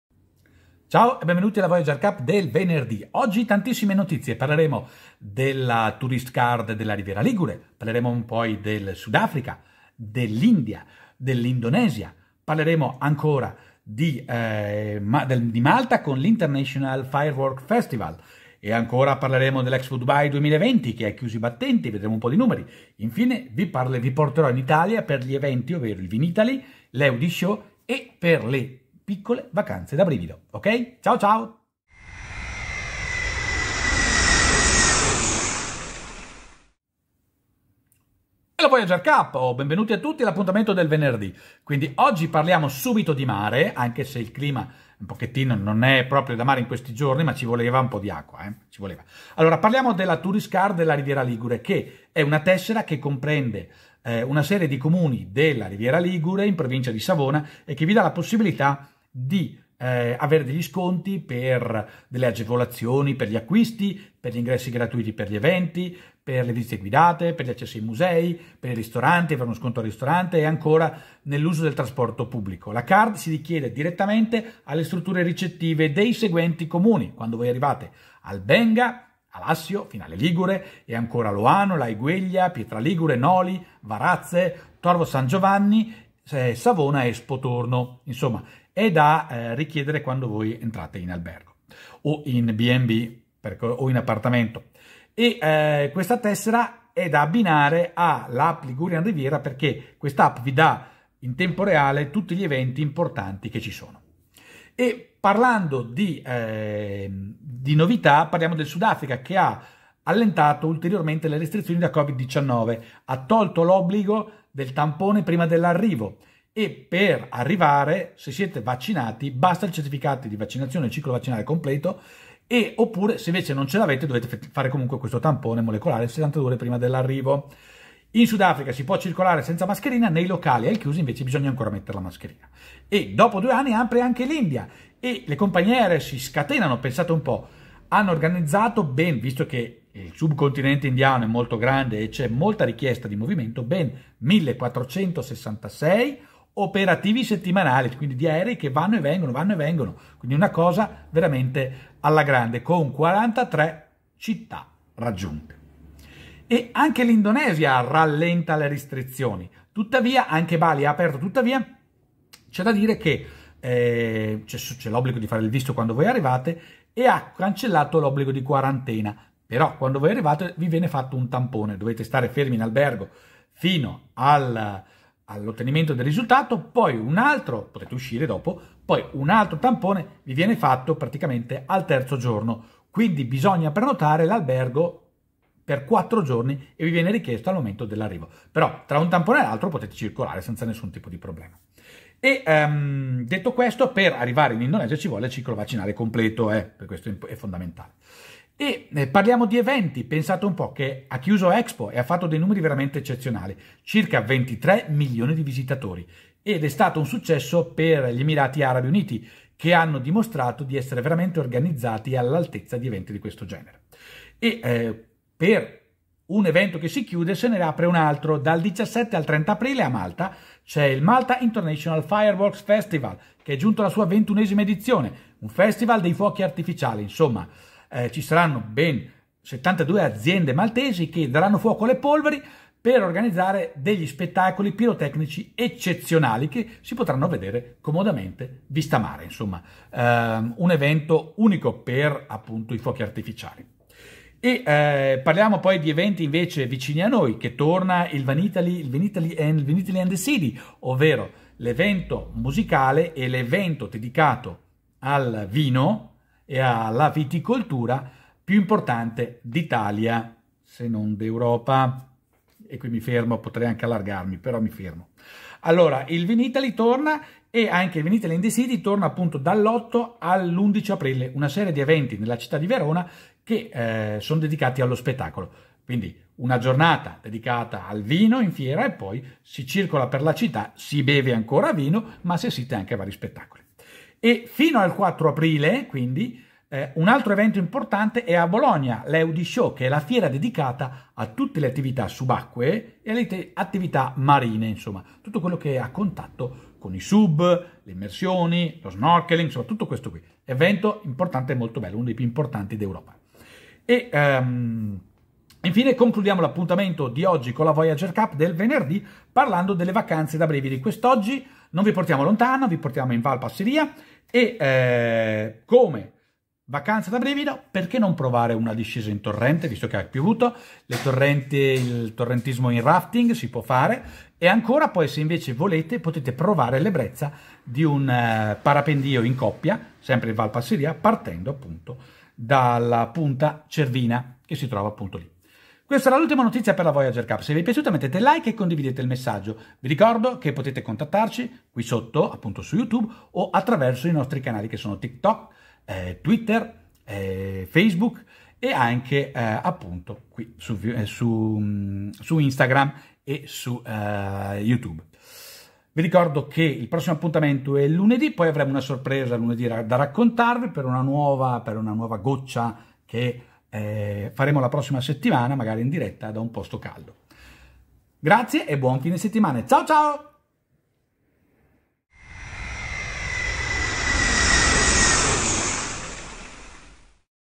Sudafrica, Indonesia, Dubai, Vinitaly, Eudishow, Turist Card Liguria e altro Voyager's Cup 01.04.22
Tante news dal mondo dei viaggi: Sudafrica senza tampone *se vaccinati, Indonesia stop quarantena, India ripartono i voli, Dubai i numeri dell'Expo 22, brividi in Val Passiria, Vinitaly ed Eudi Show in Italia.